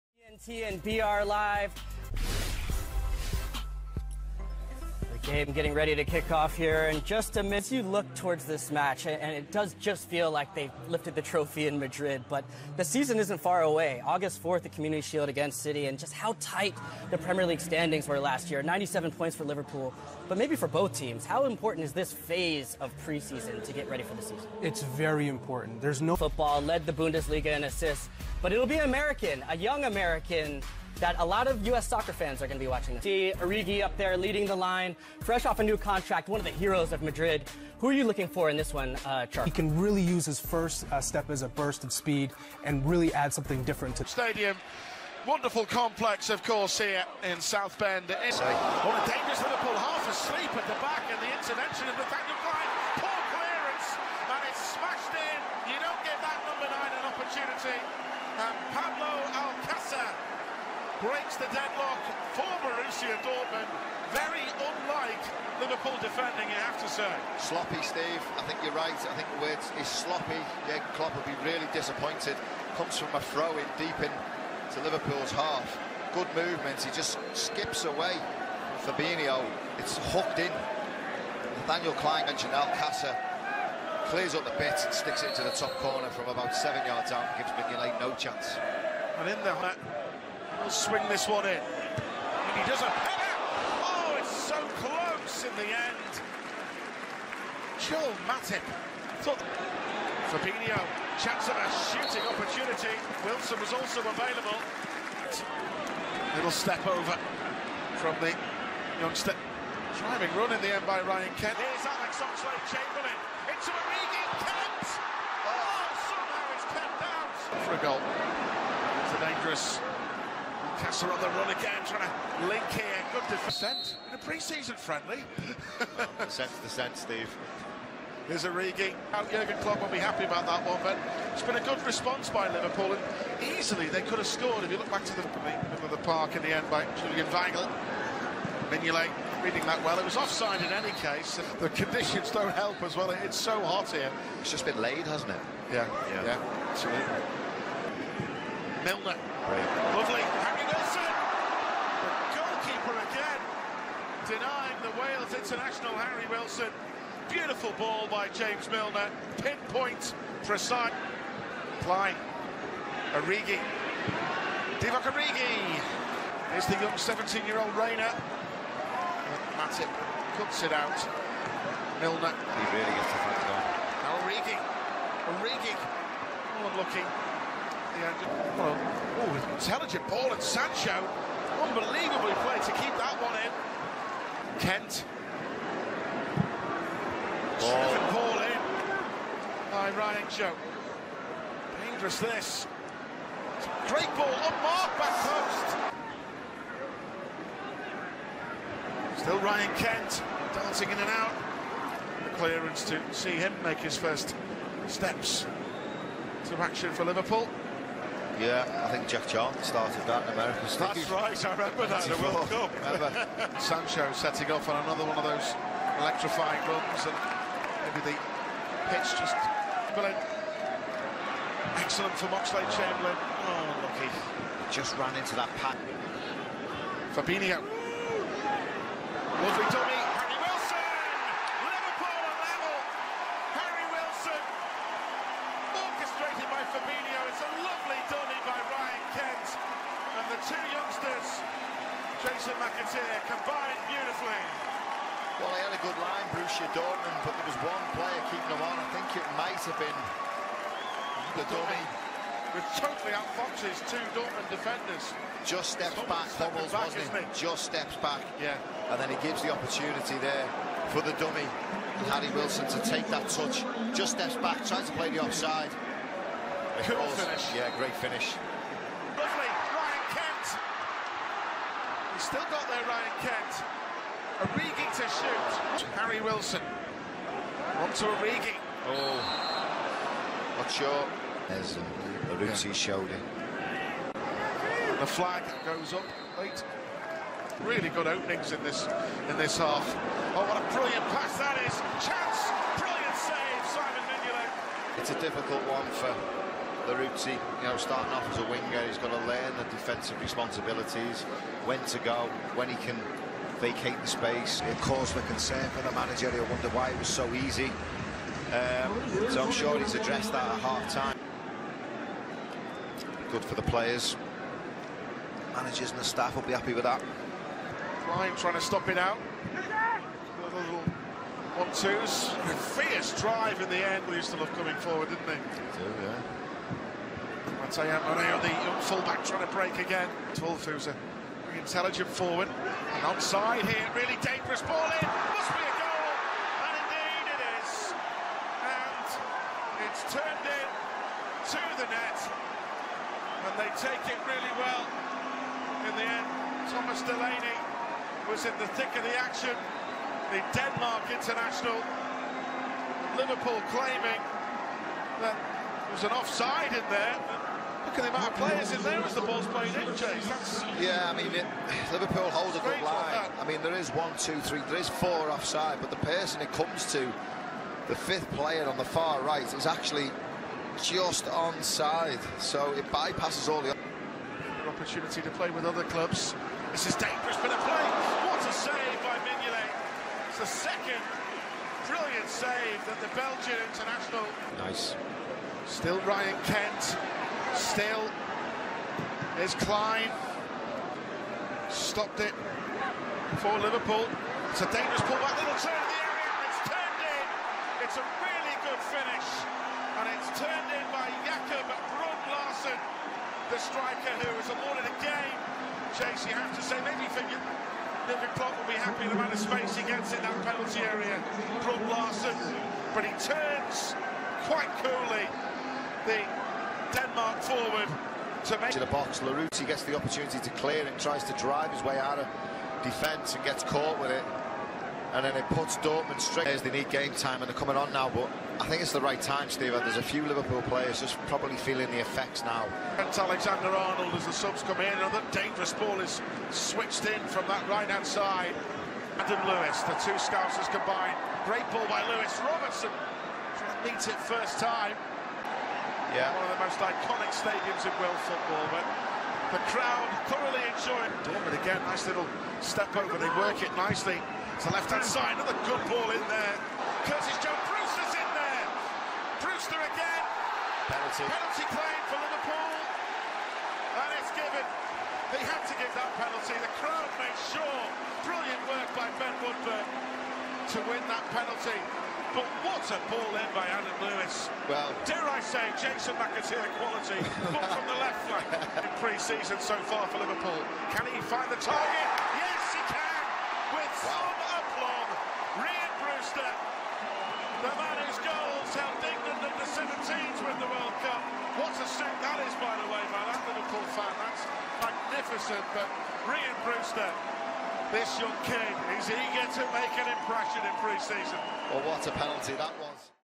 Liverpool vs Borussia Dortmund 2-3 | Highlights & All Goals | Club Friendly 2019 HD
MrFootball brings you to Real Sports - Liverpool vs Borussia Dortmund 2-3 | Highlights & All Goals | Club Friendly 2019 HD.